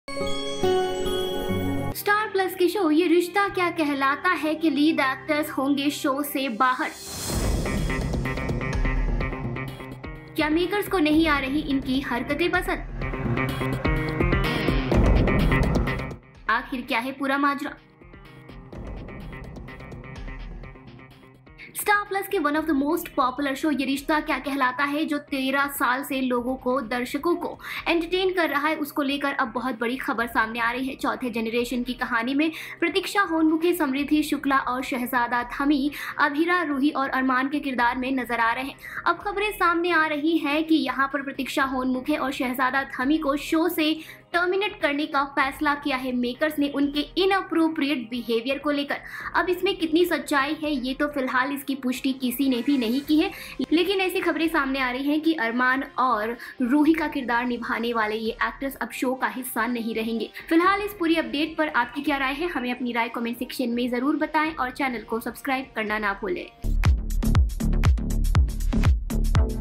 Yeh Rishta Kya Kehlata Hai: Shehzada Dhami As Armaan & Pratiksha Honmukhe As Ruhi Out From Show?
Star Plus Yeh Rishta Kya Kehlata Hai / YRKKH Update 18th March 2024: Reports are comint that producer Rajan Sahi terminated Yeh Rishta Kya Kehlata Hai actors Shehzada Dhami who plays the role of Armaan and Pratiksha Honmukhe who seen as Ruhi from the show due to inappropriate behavior on the sets of the show!!